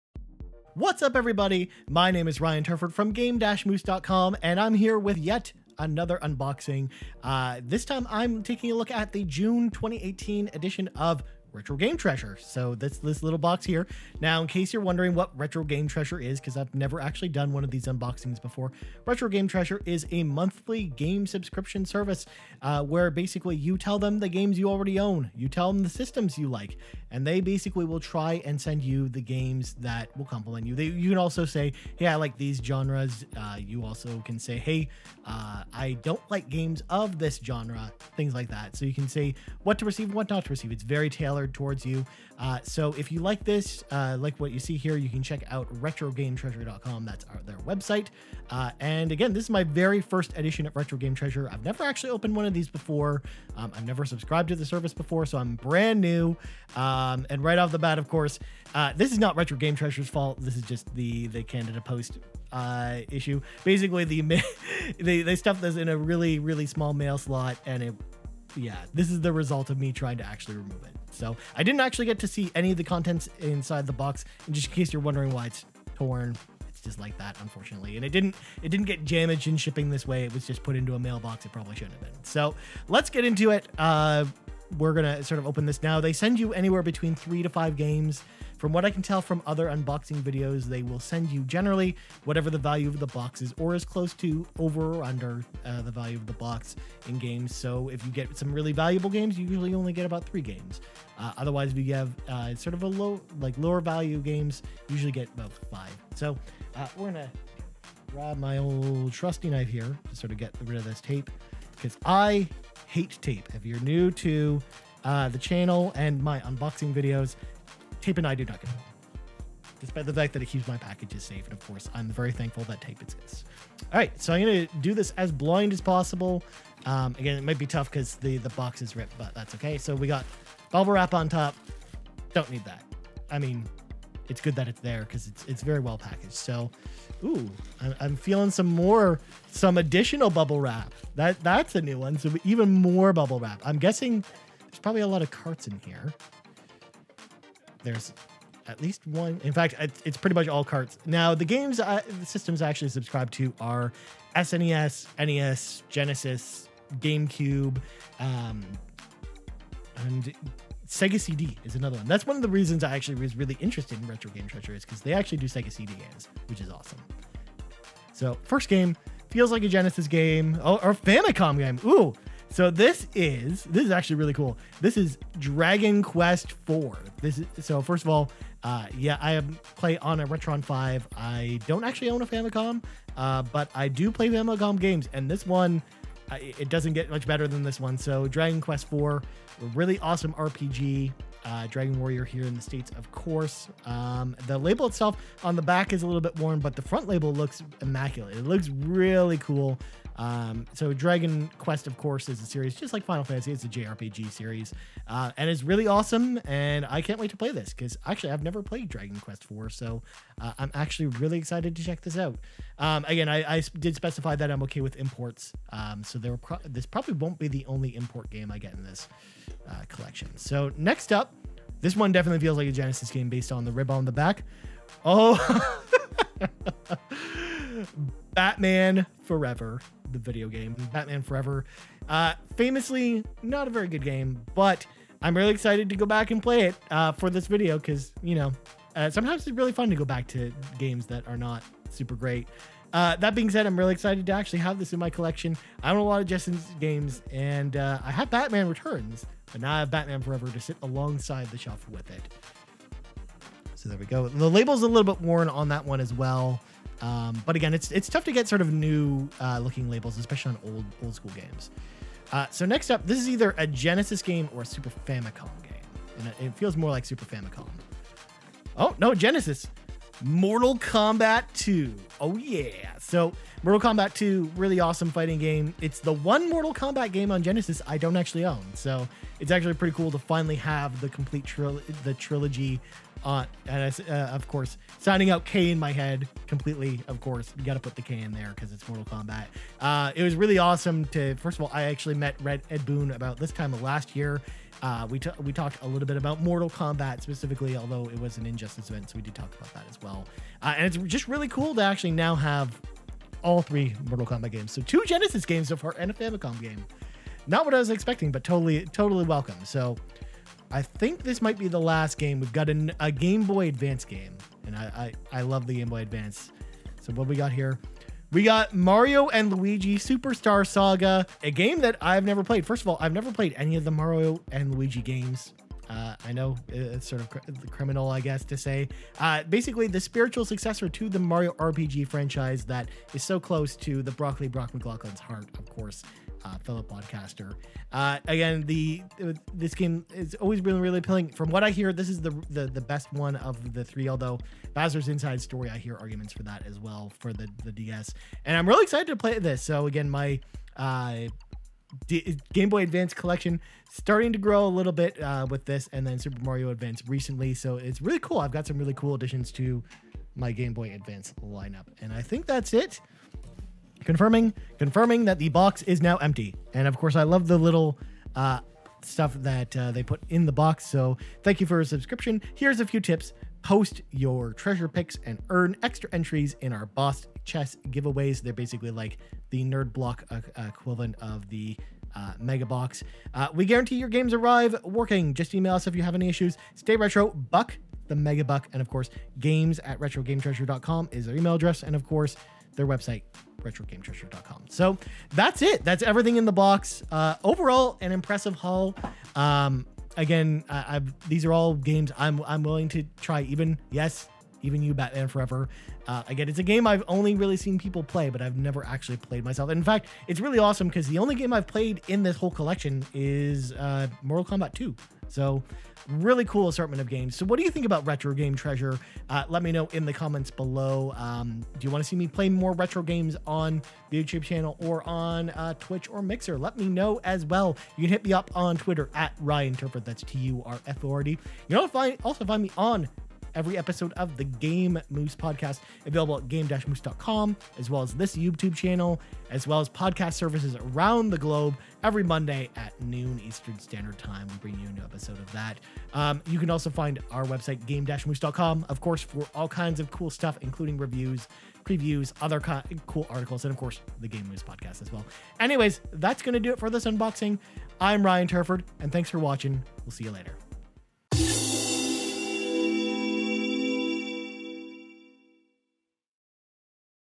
Retro Game Treasure Unboxing | June 2018 | Game Moose Unboxes!
Ryan unboxes this month's Retro Game Treasure!